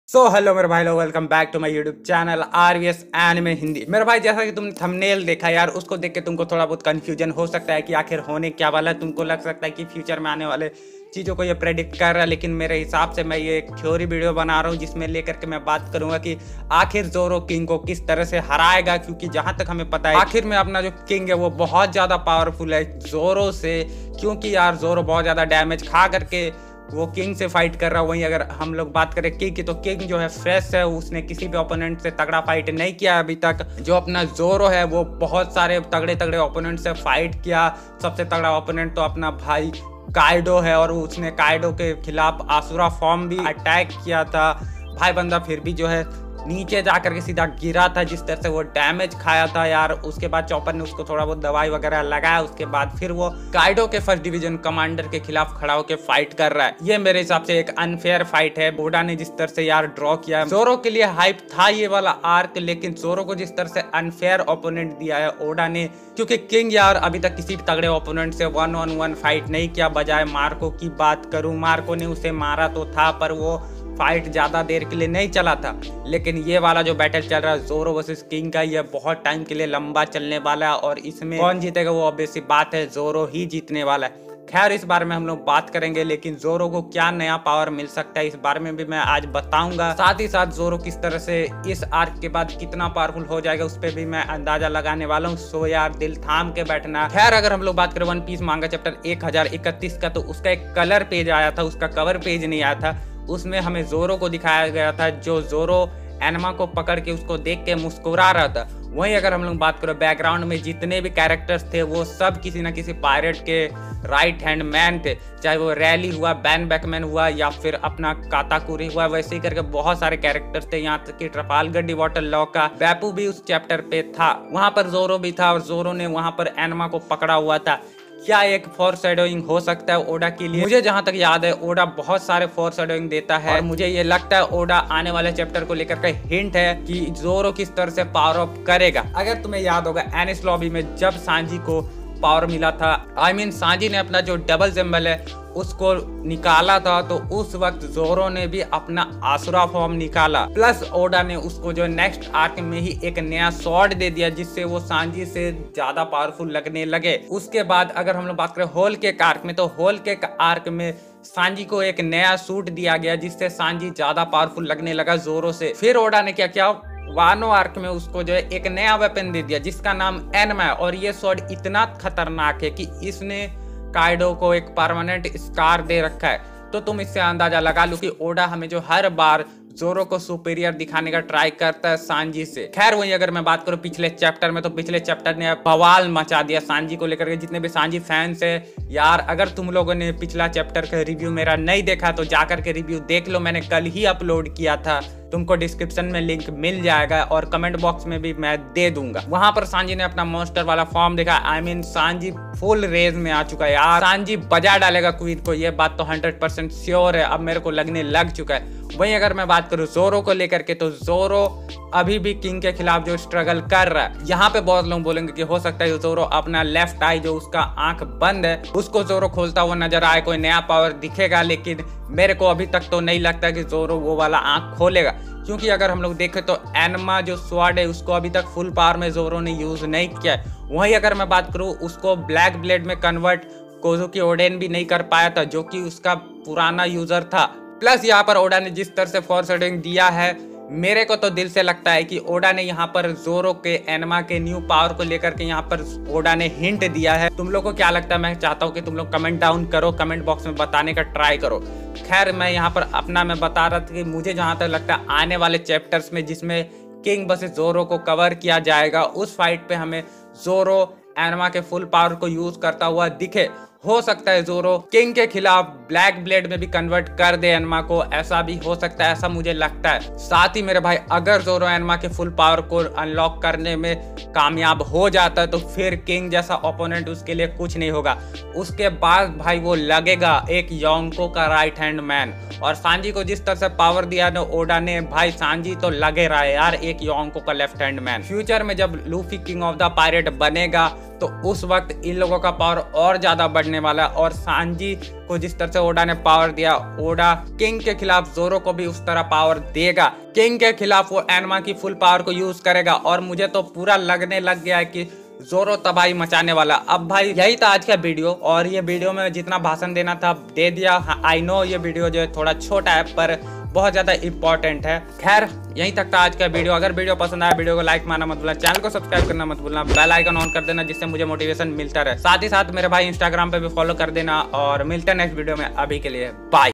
How Zoro will defeat King ? | Zoro upcoming new power explain Hindi
Yamato Devil fruit explain

Discord server:-
Thousand sunny